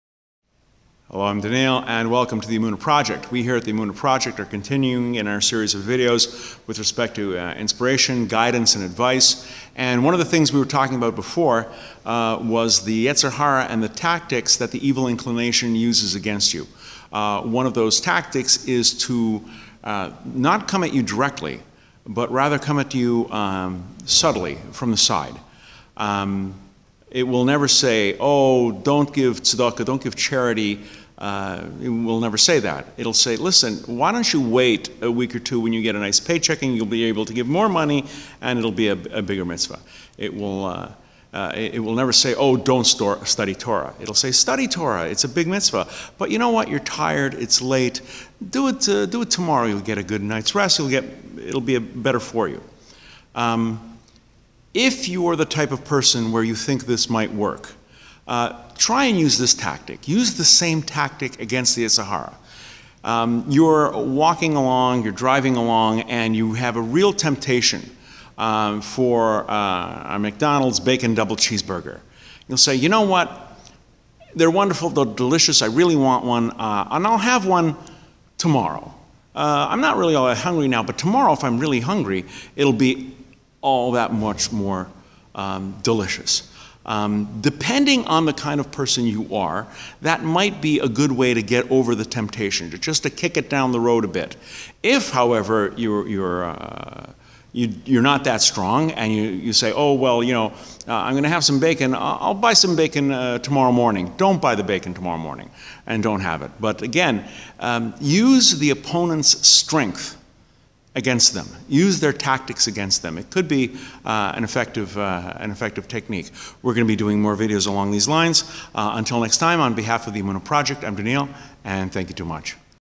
Yetzer Hara - 04 - "Tomorrow" (part 2)
Join Rav Daniel Ventresca as he speaks on a variety of topics of spiritual and practical interest to Converts/Gerim, Returnees/Baalei Tshuva and Noahides/Bnei Noach.

The Emunah Project. Yetzer HaRa. Evil inclination. Do it tomorrow.